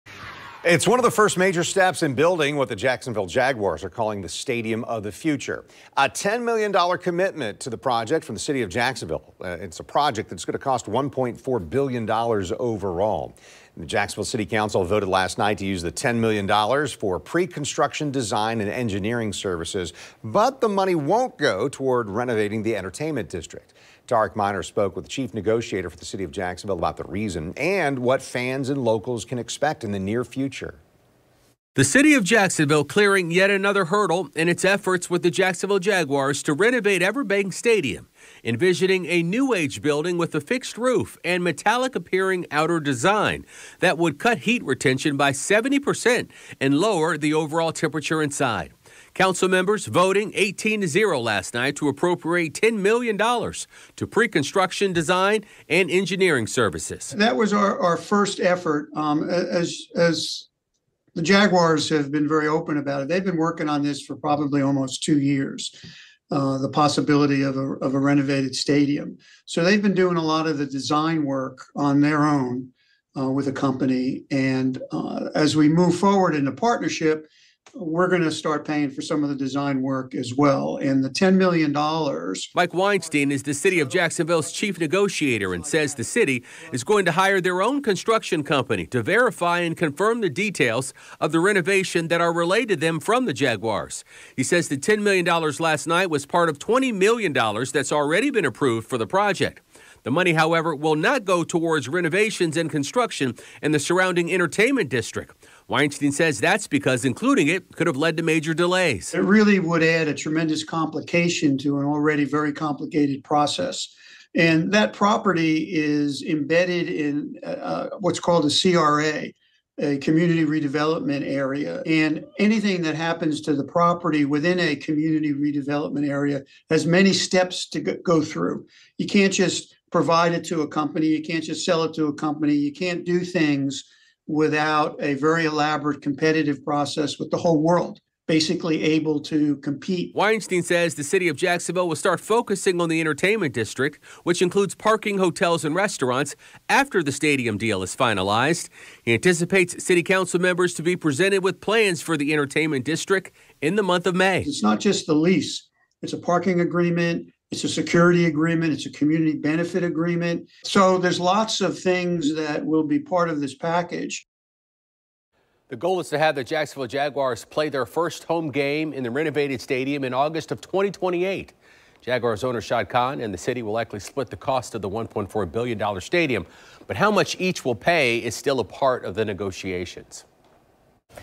City Council approves $10M for ‘Stadium of the Future’ design, engineering services Play Video
The Jaguars’ “Stadium of the Future” took a big step forward Tuesday night when the Jacksonville City Council voted in favor of a plan to spend $10 million to get started on the stadium’s construction design and engineering services.